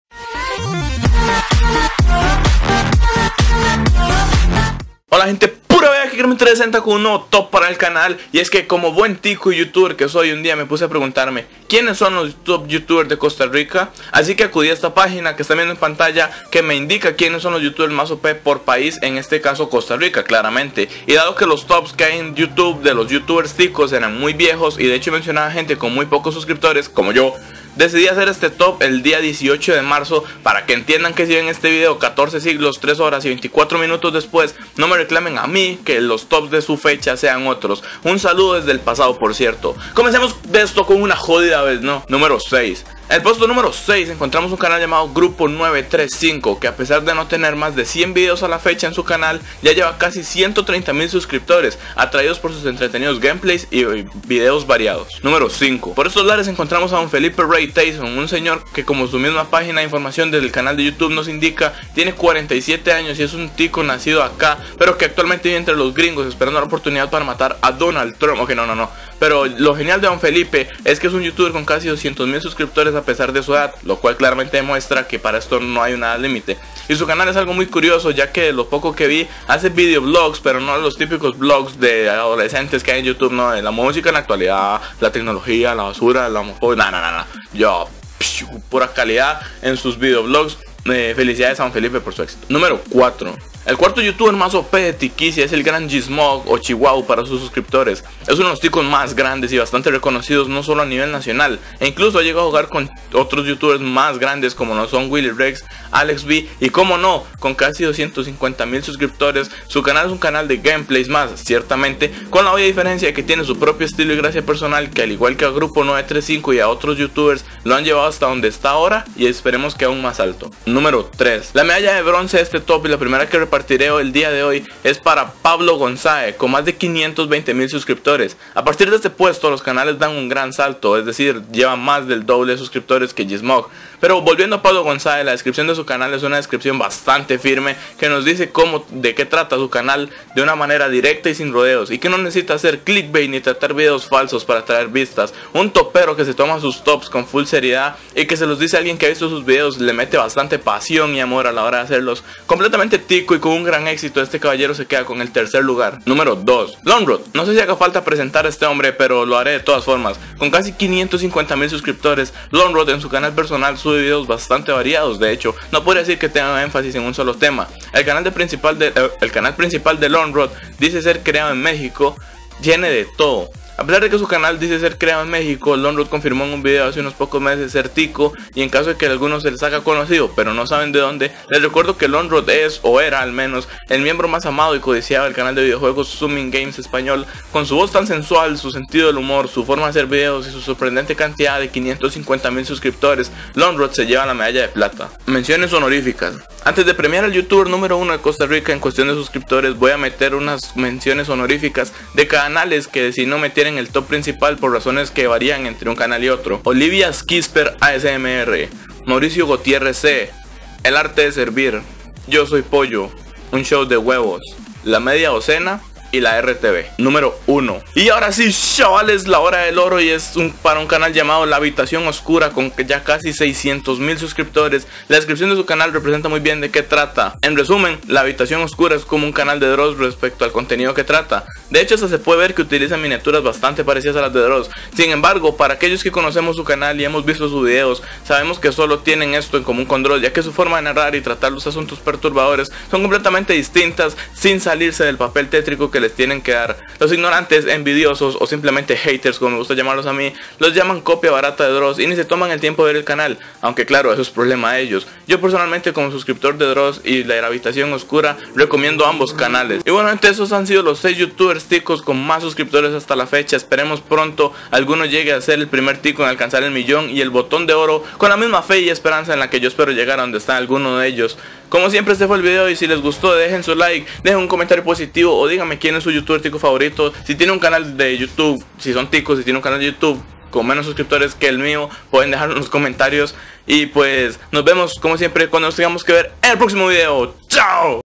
Top 6 Youtubers de Costa Rica ( Los youtubers más grandes de Costa Rica )
Hola Gente Pura Vida!!! Aqui KERVIN 360 con un video muy especial que hacia tiempo quería hacer

Unete a Pokemon Costa Rica y di que vienes por mi